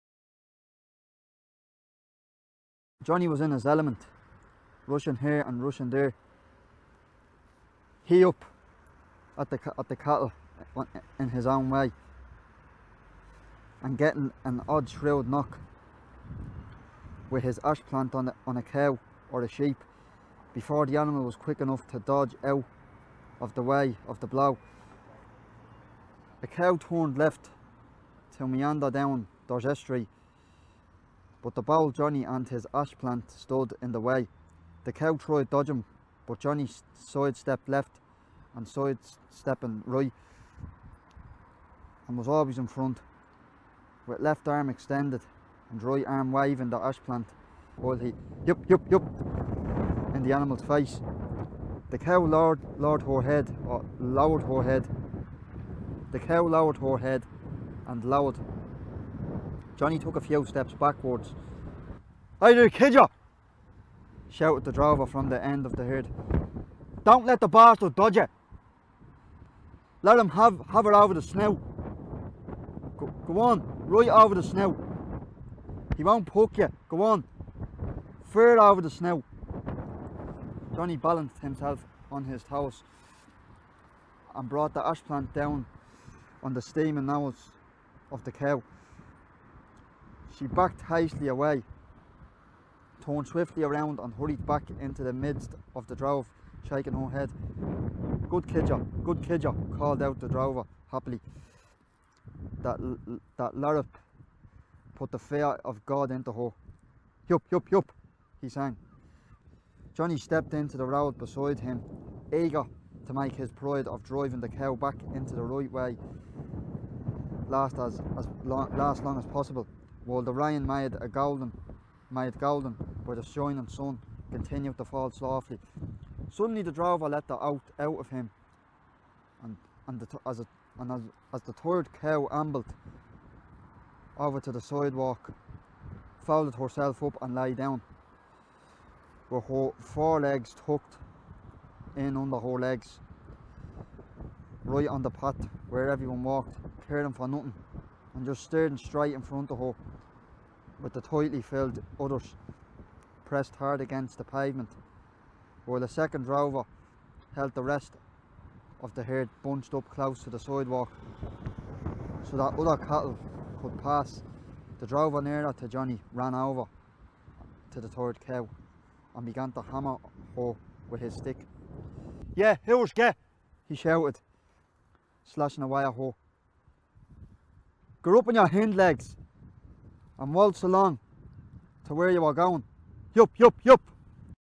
"The tired cow" by Sean O'Casey
Read by Aaron Connolly at Spencer Dock .

Part of the “O’Casey in the estate- Ploughing on” collection, created for Culture Night 2020.

The two-part documentary “O’Casey in the Estate” on RTE followed plans to stage a production of ‘The Plough and the Stars” using a non-professional cast. Sadly their once in a lifetime opportunity to step onto the Abbey Theatre stage was cancelled due to the COVID-19 pandemic.

But nothing can hold this cast back –cast members from the documentary were reunited to create this video collection.

A perfect combination of evocative memoir, passionate performers, and iconic locations, bringing the words of one of Dublin Docklands most famous figures to life.

(In conjunction with the Short Stories & Tall Tales series and the Sean O'Casey Festival 2020)

Videography by Louis Maxwell